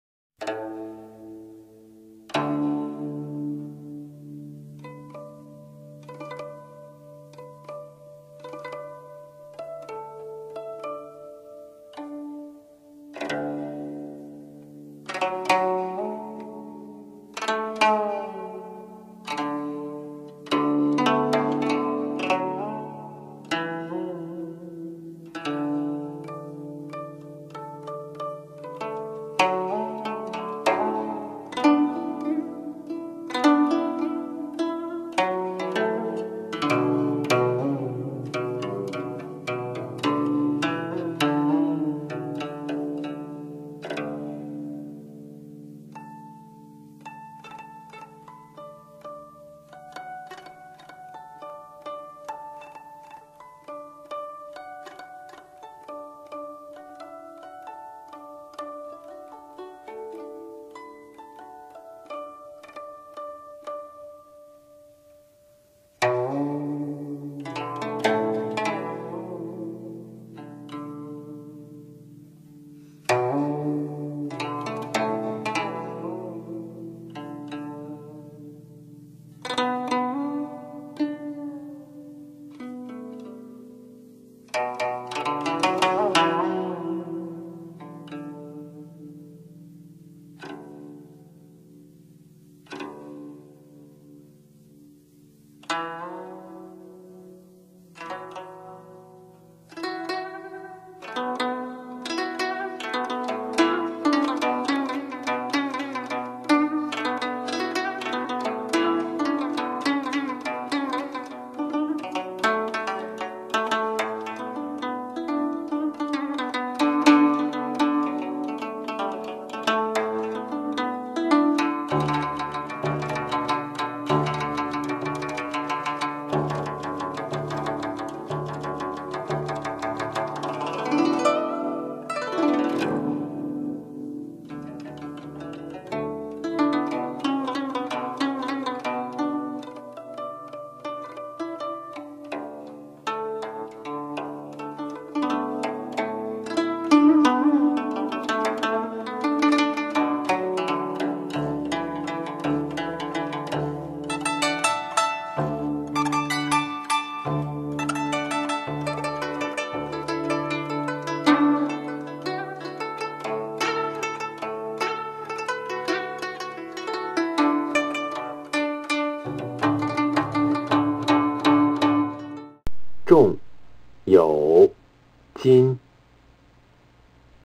Li Shu 隸書 Chinese Calligraphy Sample Characters 重有金
Sample characters (not a phrase):
重 heavy [zhong4] / repeat [chong2]
有 have; has
金 gold; a Chinese surname

Filename: RED-LiShu-HeavyHaveGold-SPEED125.wmv